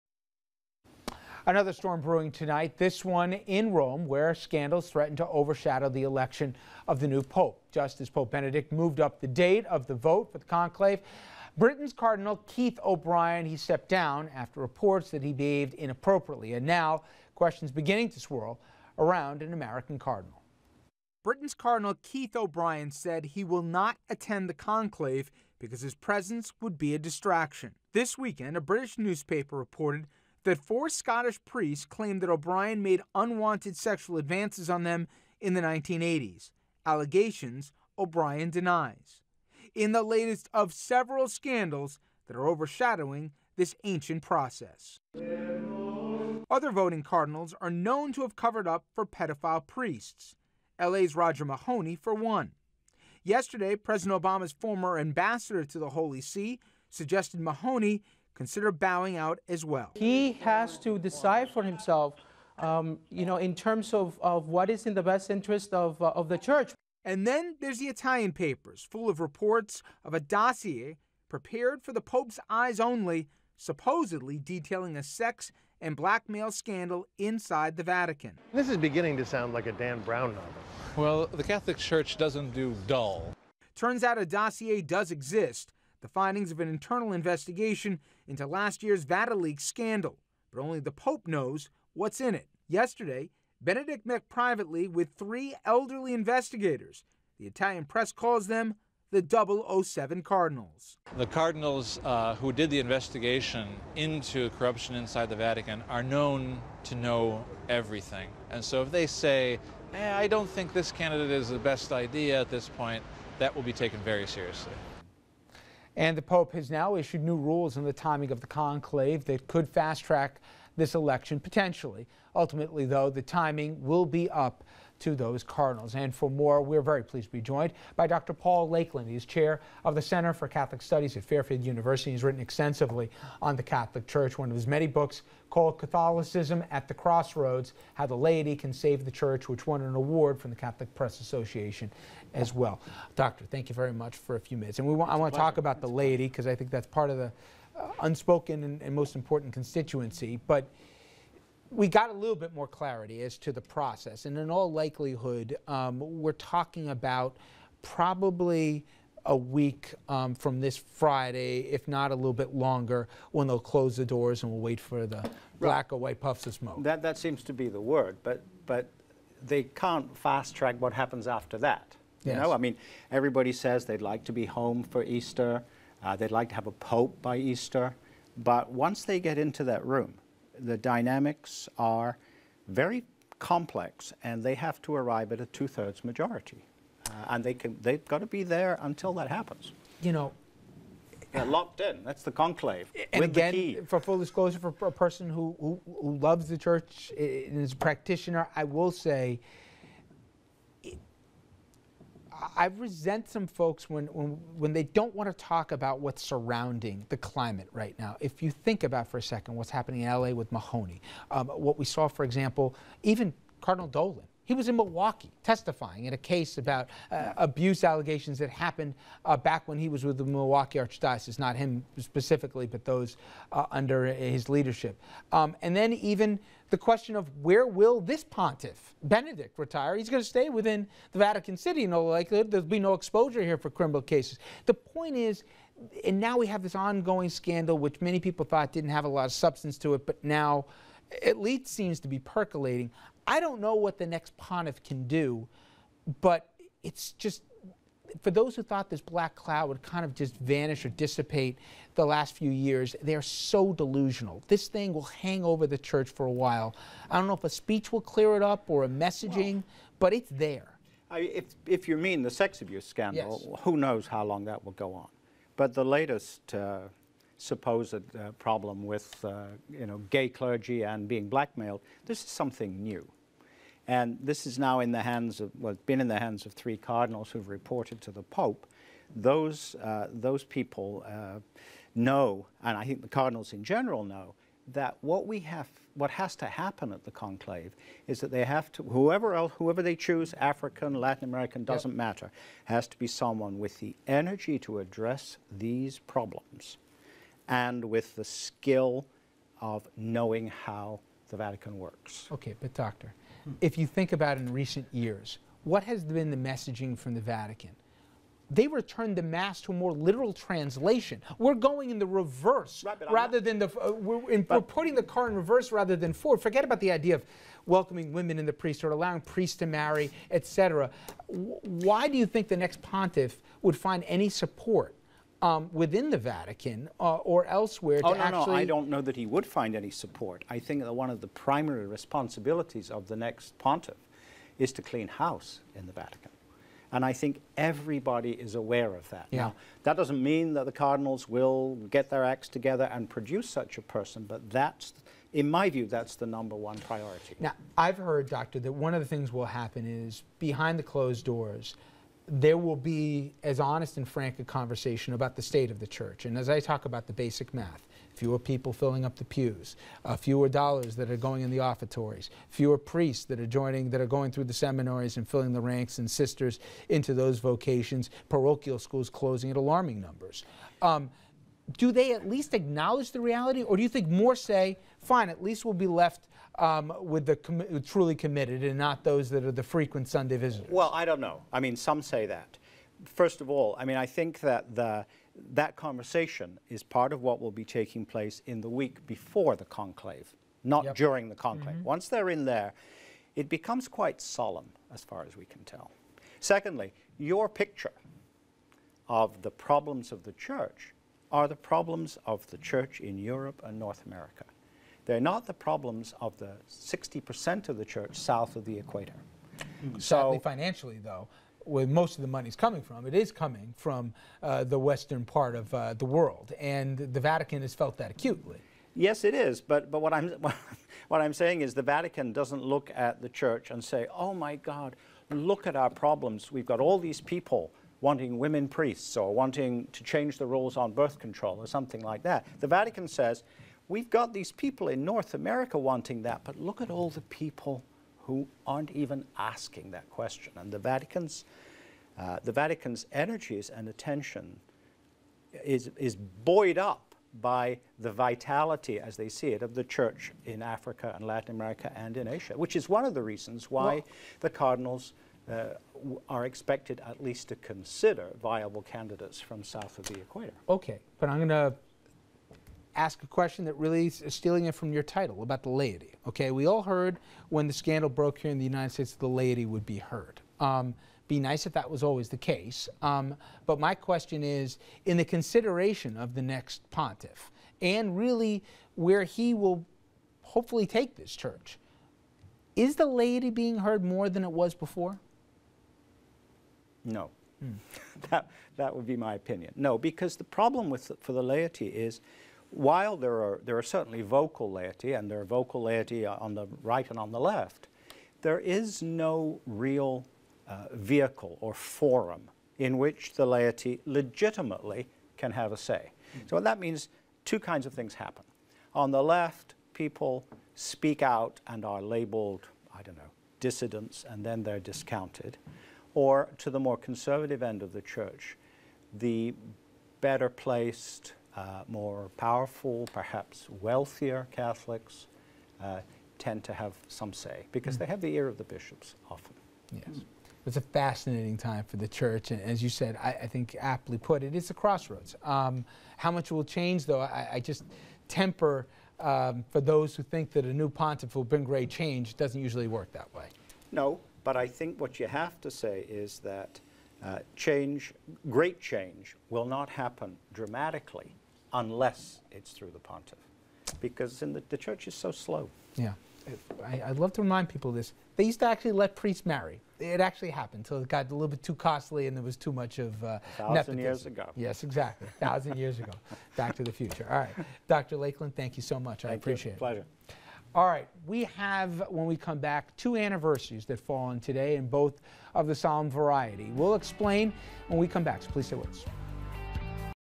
The Vatican at a Cross Roads: Scandal Threatens to Overshadow Pope's Last Days
A new scandal is threatening to overshadow the election of the new pope as Pope Benedict XVI is about to step down. Over the weekend, a newspaper reported that four Scottish priests claim that Keith Cardinal O'Brian made sexual advances toward them, claims that he denies.